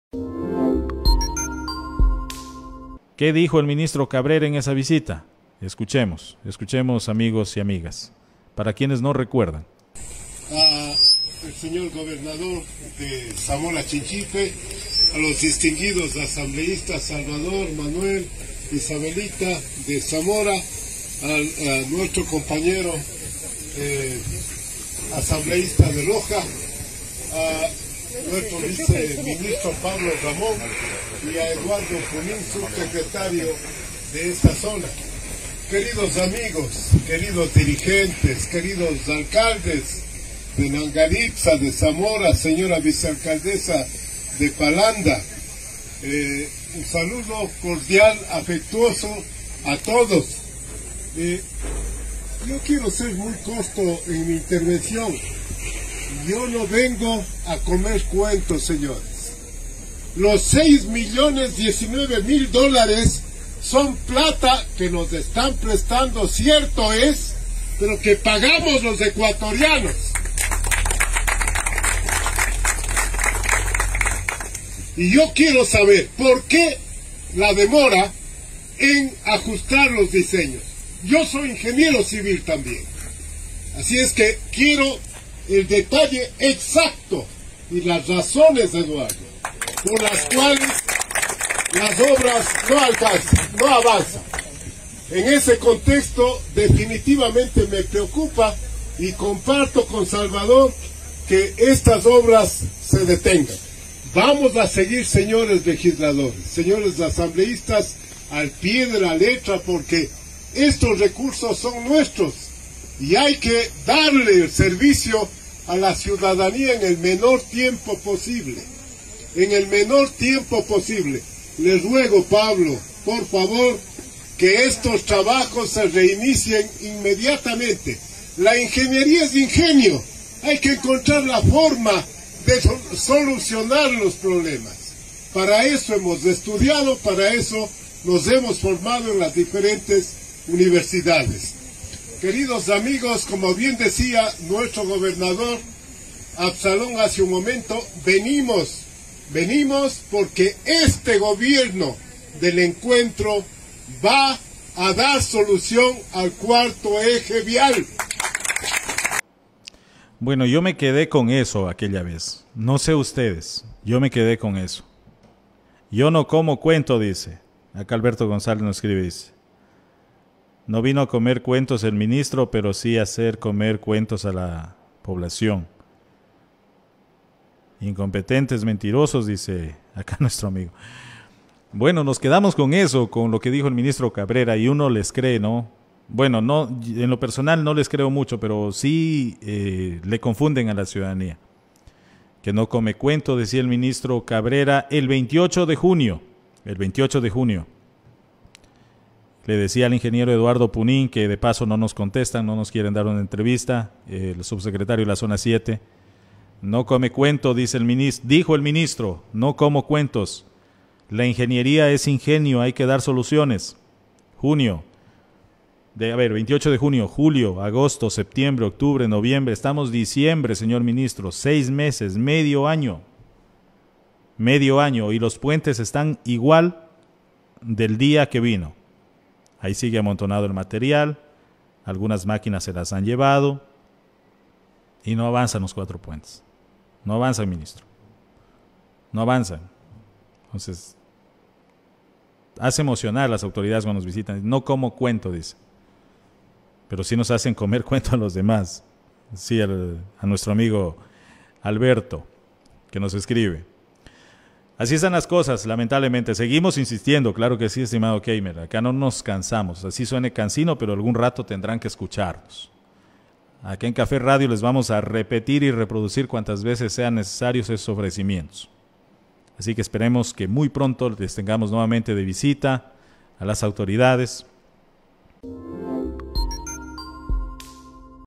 Disposición incumplida del Ministro Marcelo Cabrera - IV Eje Vial
Hace seis meses el Ministro de MTOP, Marcelo Cabrera, dijo que "no come cuento" por la demora en actualización de los estudios para la construcción de 4 puentes en el IV Eje Vial, su disposición inmediata lleva medio año incumplida.